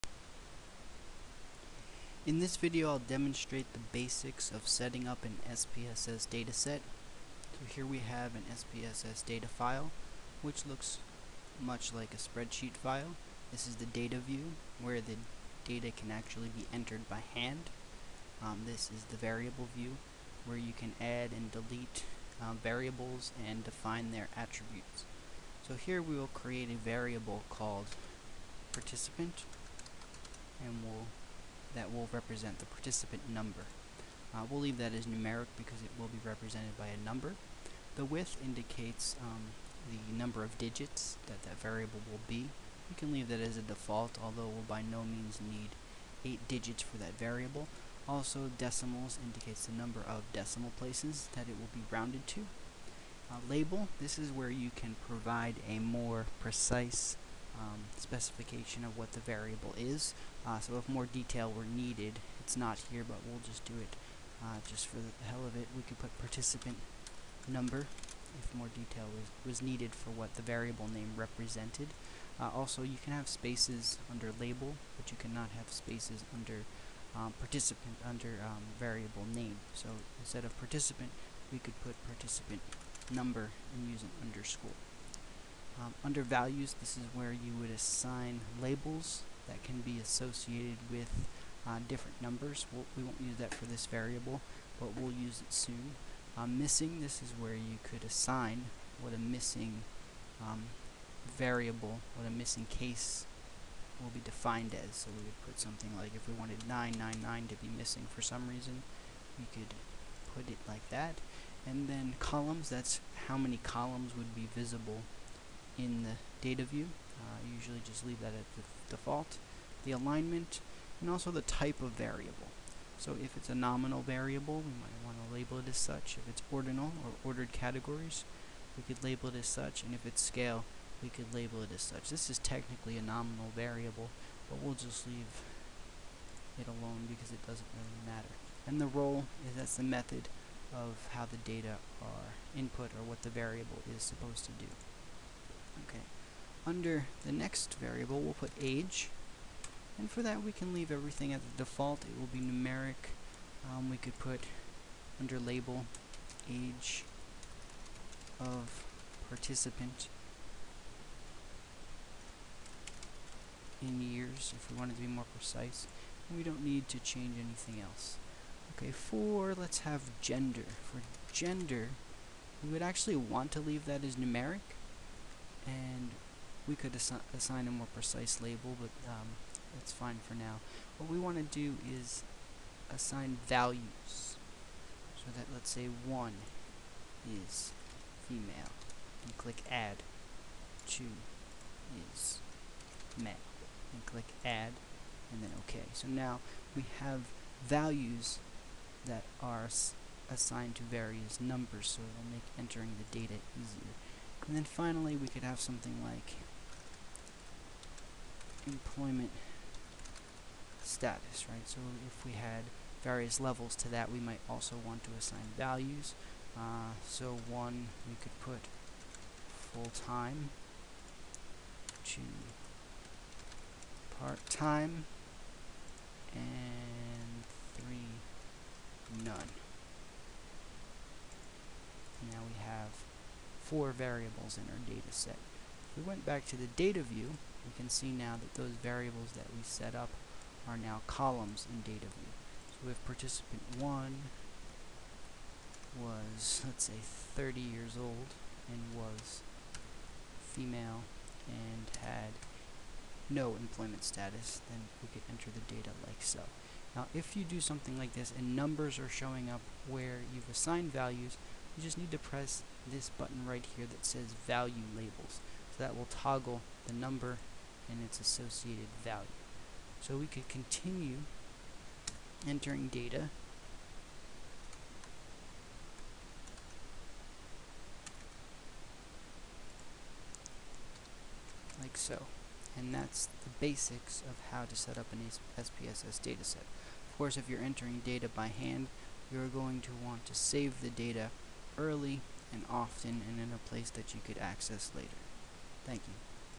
Setting Up Variables in an SPSS Data Set
This video shows how to setup variables in an SPSS data set.